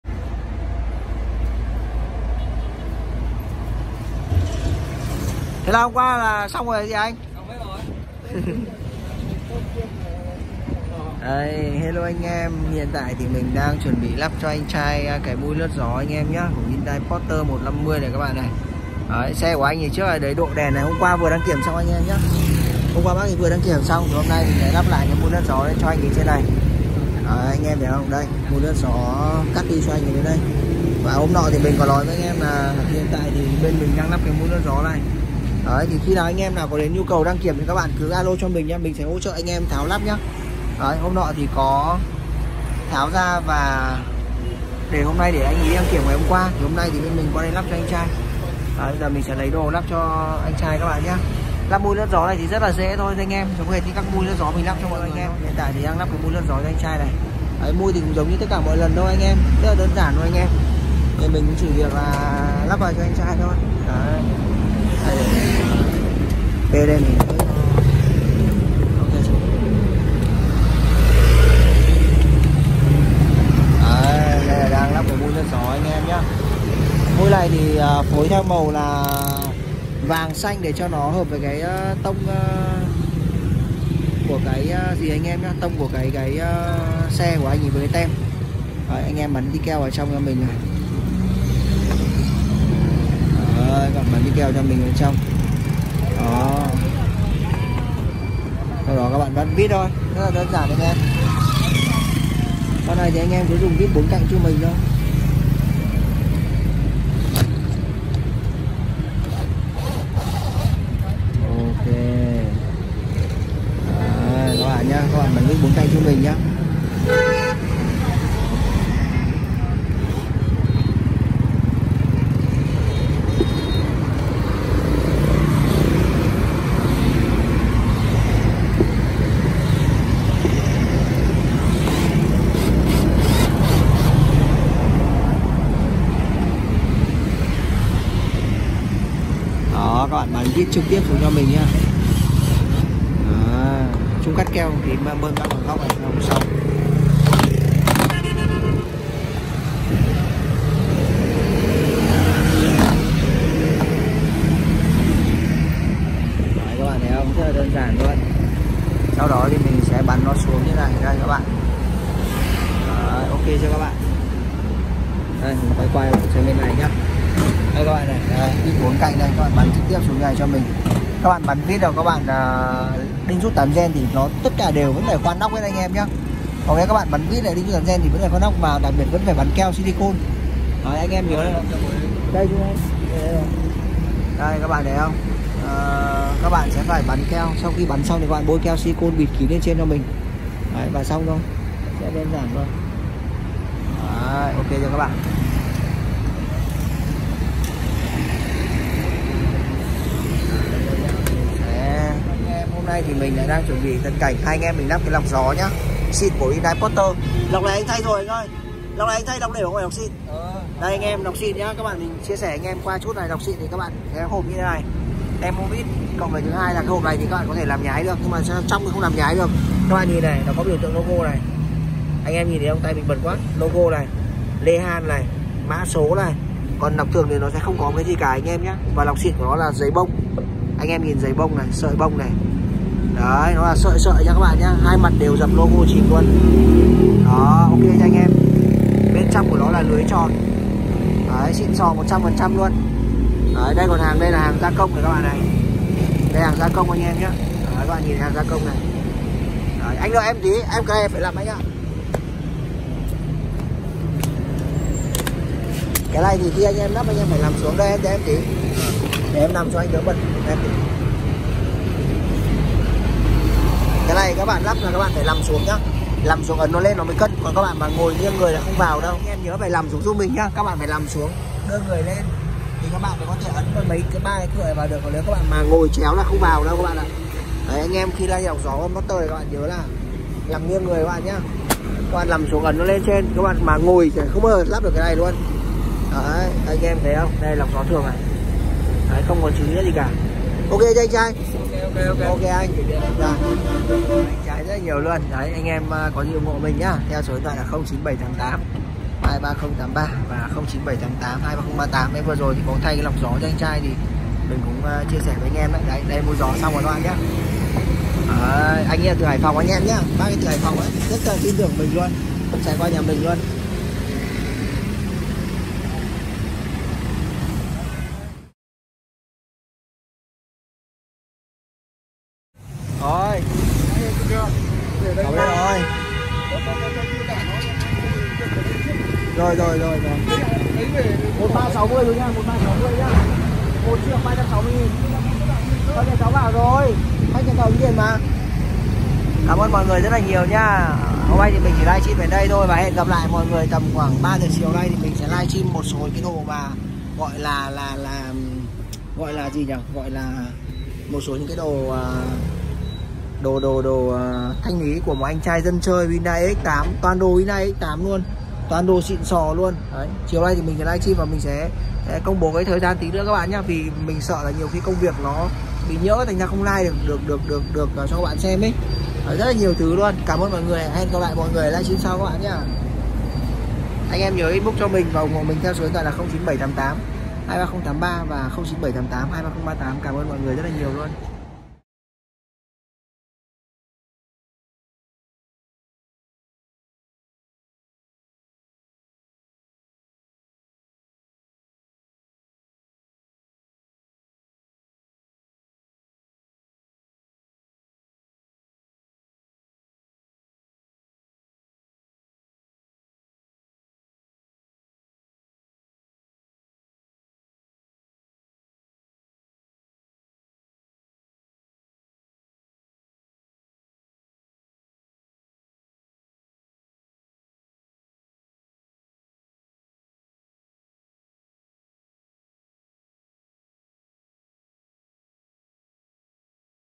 Review lắp mui nướt gió xe huyndai Poter H150 sau lần đăng kiểm đầu tiên , anh trai quảng ninh
phutungoto huyndai kia izusu phutungotonhatduc dochoix. Á